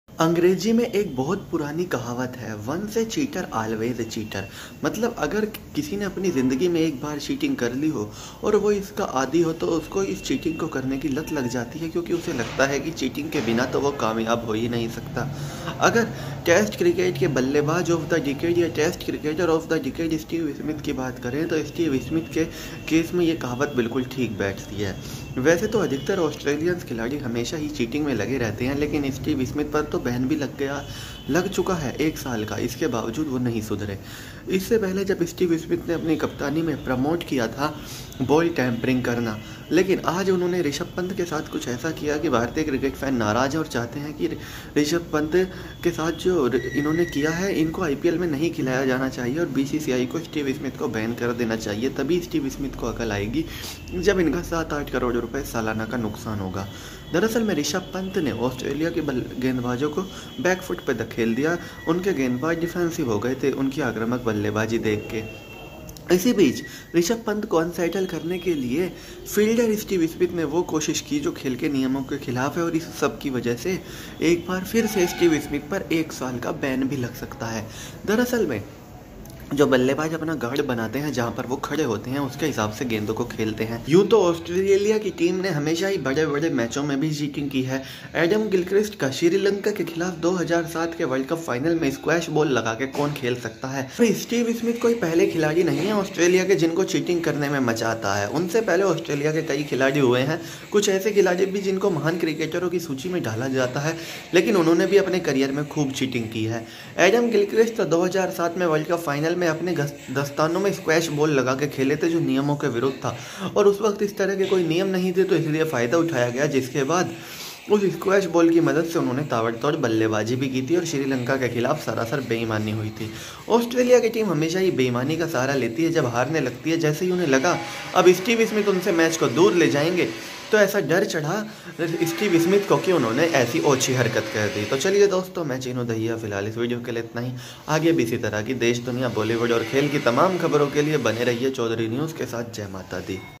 Steve Smith - Once a cheater always a cheater | Rishabhpant ,Steve smith scuff
Steve smith scuffed the guard of rishabh pant.
steve smith is a cheater.